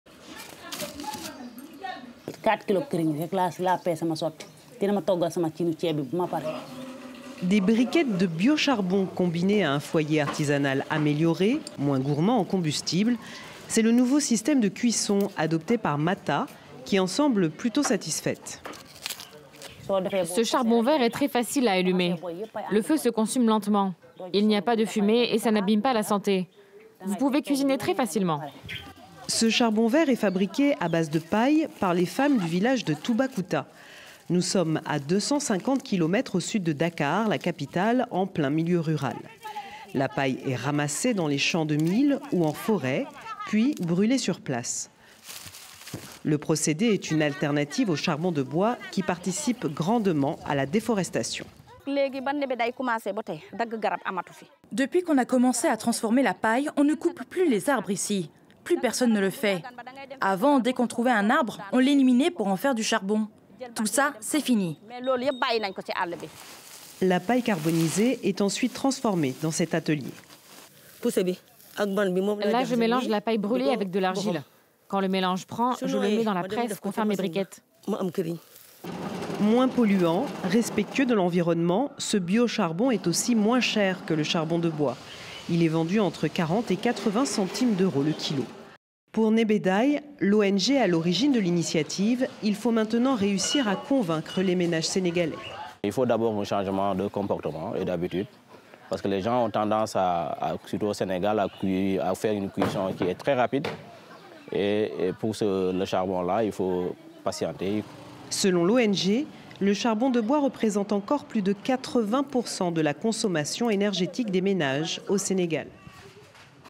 Sénégal : le bio-charbon, un fioul vert pour protéger les forêts • FRANCE 24
110 millions d'hectares de forêts sont menacées par les effets de la cuisson, notamment au charbon de bois, en Afrique. Comme solution, l'association sénégalaise Nebeday fabrique depuis plus de 10 ans déjà du charbon bio à base de paille qui permet de réduire les besoins en bois de chauffe des ménages de plus de moitié. Reportage d'Aminatou Diallo et Sarah Sakho pour France 24.